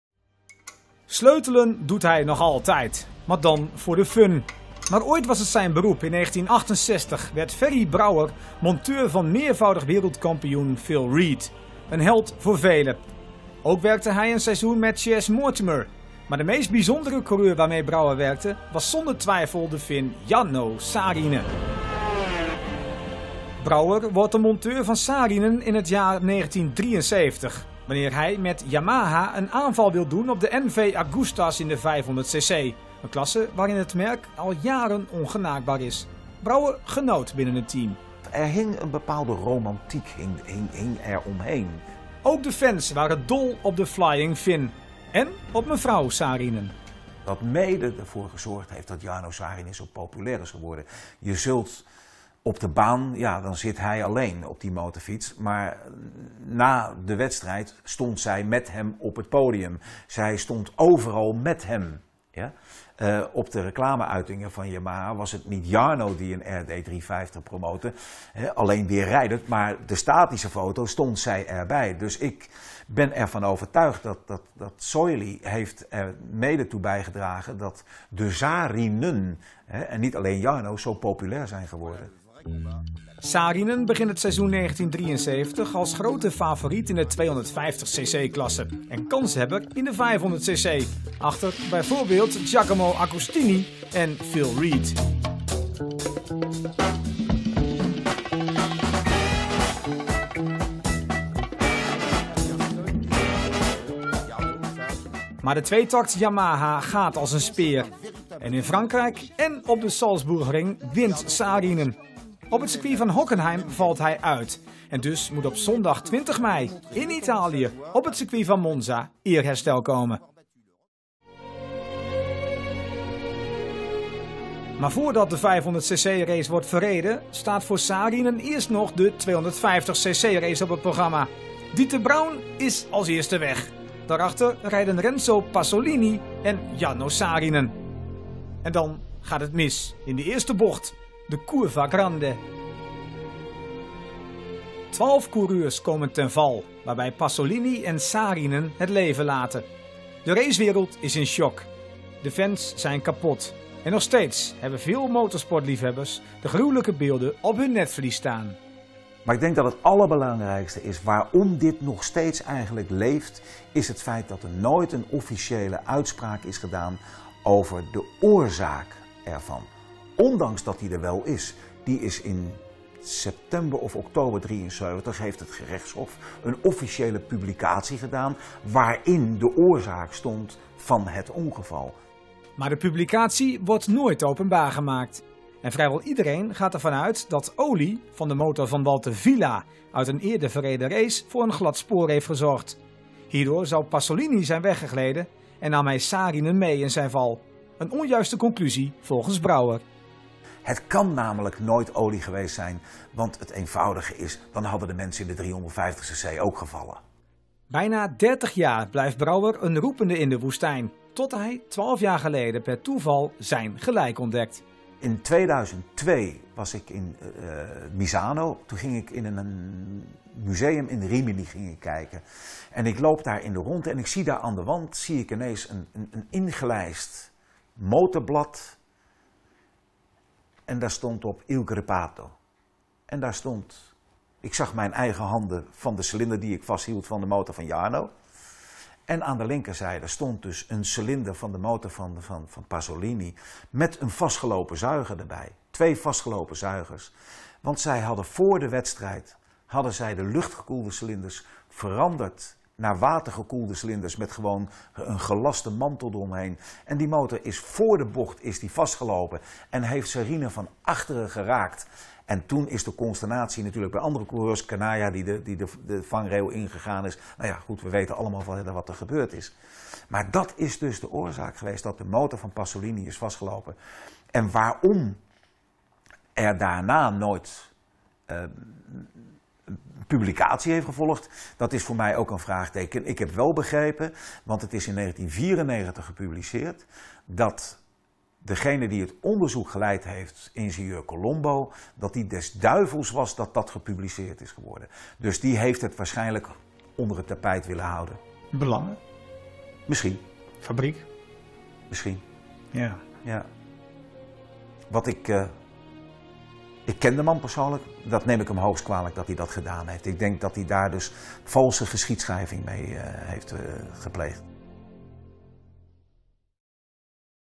Ferry Brouwer: Olie niet de reden van dodelijke crash Jarno Saarinen
MOTORSPORT - Velen kennen Ferry Brouwer als de man achter het succes van de Arai-helmen in Europa of van de Centennial TT in 1998, of als animator van het Yamaha Classic Racing Team. Maar de inmiddels 65-jarige geboren Hagenaar was ooit monteur van de allergrootste coureurs ter wereld.

De vrouw van Saarinen
Brouwer was bijna drie jaar lang de vaste monteur van meervoudig wereldkampioen Phil Read (woonde zelfs twee jaar bij hem en diens vrouw in huis), één seizoen van Chas Mortimer (in 1972, na een ruzie met Read) en in 1973 hielp hij de populaire Fin Jarno Saarinen, die een seizoen daarvoor wereldkampioen in de 250cc was geworden. "Werken met Jarno was geweldig. Er hing een bepaalde romantiek om die man heen. Hij was zeer populair bij het publiek. Al kwam dat ook voor een belangrijk deel door Soili, de appetijtelijke vrouw van Saarinen", aldus Brouwer. "Overal waar hij was, daar was zij ook. Voor en na de race, zelfs tot op het podium. Zelfs Yamaha adverteerde steevast met haar op de foto."

Aanval op ongenaakbare MV Agusta
Saarinen had in 1973 twee doelen. Hij wilde zijn wereldtitel in de 250cc prolongeren en een gooi doen naar het kampioenschap in de 500cc, de klasse die al jaren werd gedomineerd door MV Agusta (met coureur Giacomo Agostini). Het seizoen begint fantastisch. Op Paul Ricard en op het circuit van de Salzburgring in Oostenrijk pakt de 'flying Fin' de dubbel. Op Hockenheim wint hij de 250cc race, maar valt hij uit in de koningsklasse. Op 20 mei moest in Italië eerherstel volgen, op het circuit van Monza.

Tragisch ongeval
Zoals gewoonlijk moet Saarinen eerst de 250cc race rijden, de vierde race van de dag. Dieter Braun is als eerste weg bij de start, gevolgd door Renzo Pasolini en Saarinen. Bij de eerste bocht gaat het gruwelijk mis. Twaalf coureurs komen ten val. Pasolini en Saarinen overleven de crash niet.

Oliespoor Walter Villa
Over de oorzaak van de dood van de Italiaan en Fin is nooit een uitspraak gedaan. "In september heeft het Gerechtshof wel een officiële publicatie gedaan, maar die is nooit openbaar gemaakt. Daardoor bleef vrijwel iedereen, ook de vrouw van Jarno, geloven dat het oliespoor van Walter Villa (gemorst in de daarvoor gereden 350cc race) ervoor zogde dat Pasolini ten val kwam met alle gevolgen van dien."

Brouwer twijfelt meteen aan die lezing en krijgt de bevestiging van zijn eigen bevindingen 39 jaar na het fatale ongeluk, in een museum in het Italiaanse Rimini. Een journalist achterhaalt de ware toedracht. In de reportage van TV Drenthe vertelt Ferry Brouwer zijn verhaal. "Ik heb later begrepen dat ingenieur Colombo, die verantwoordelijk was voor het onderzoek, des duivels was dat de journalist het publiceerde. Kennelijk wilde hij het ware verhaal 'onder het tapijt' houden. Ik ken de man persoonlijk en dat neem ik hem hoogst kwalijk."